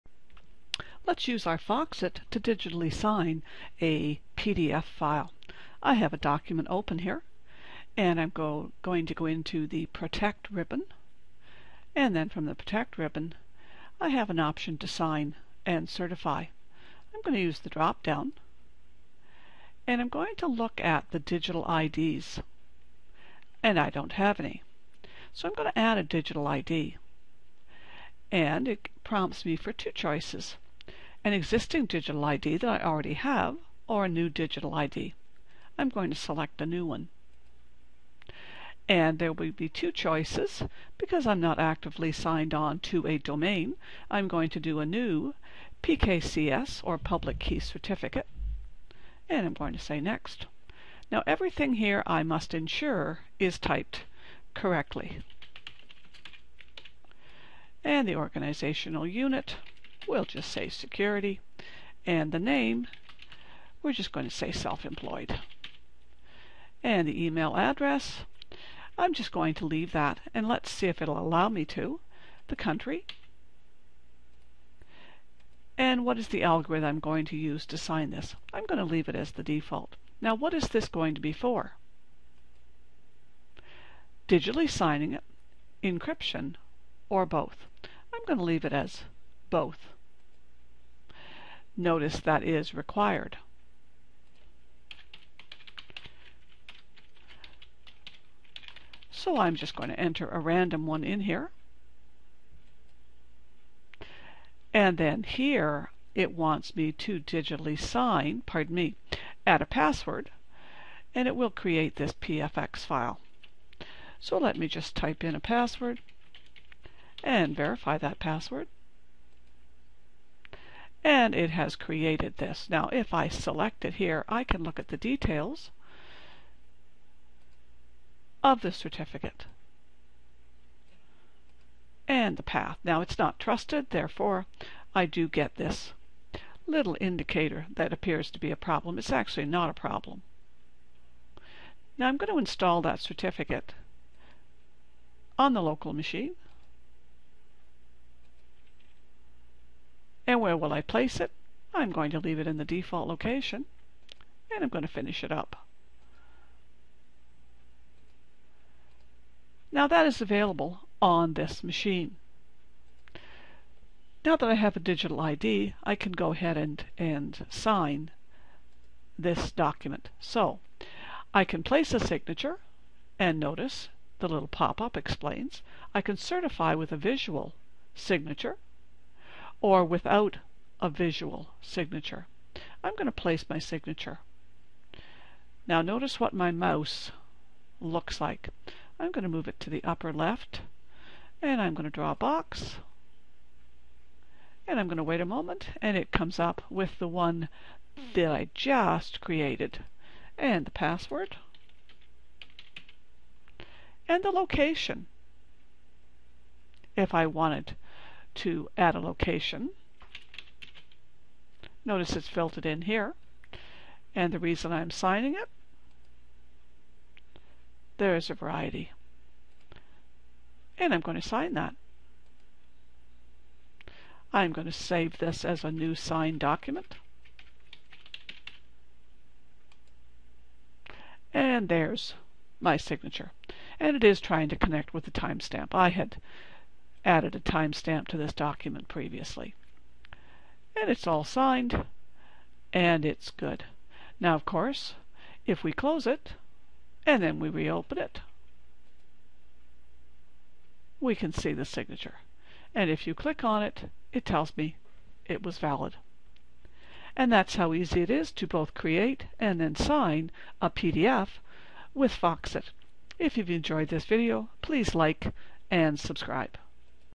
Foxit Create a Digital Signature and Sign a PDF
Learning Foxit.  Learn how to create a NEW DIGITAL ID and then apply it to a PDF file.